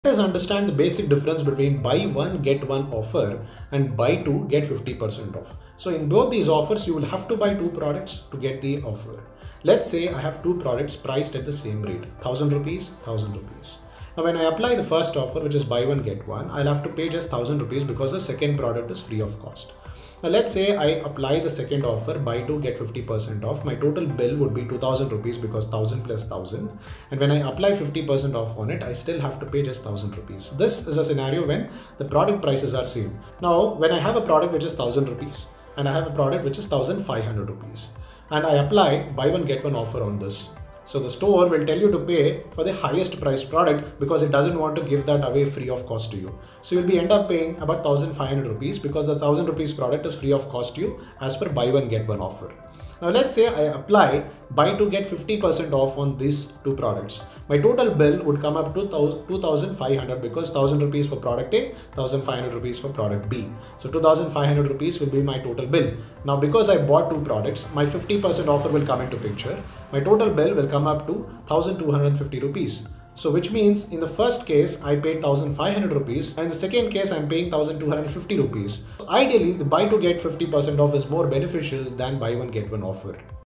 How to pay less for your next shopping purchase?
Offers are interesting and they always lure us to buy more.

while we shop from lifestyle stores near us or shopping online, understanding offers can save us money

Here is how to do it